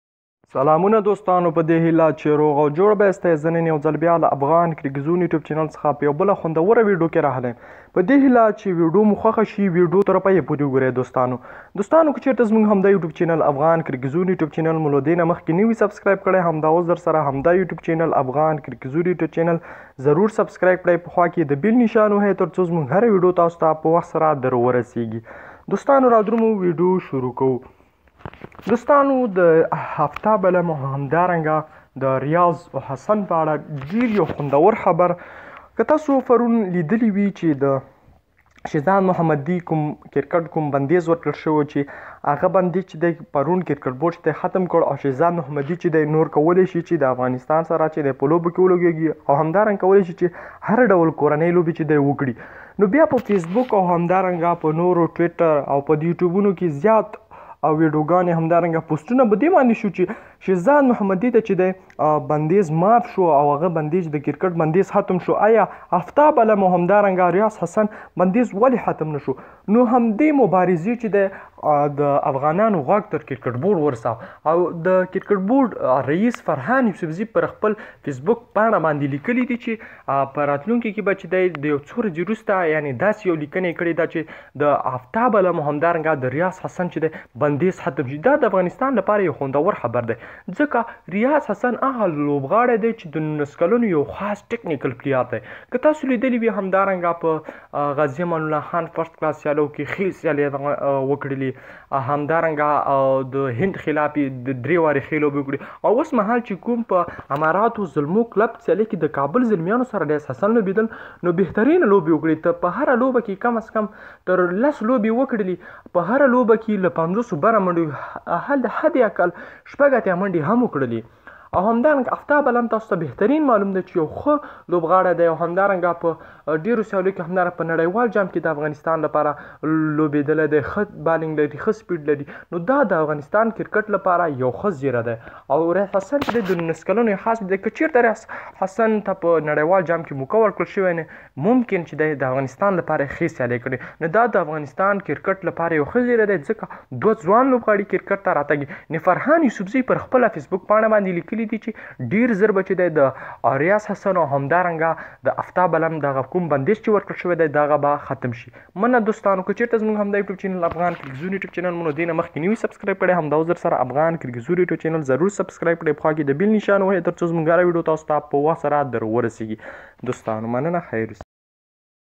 د افغانستان کرکټ ، افتاب عالم او ریاض خسن لپاره خوندور زیری دلته دغه ویډیو کې وګورئ.
Afghan cric zone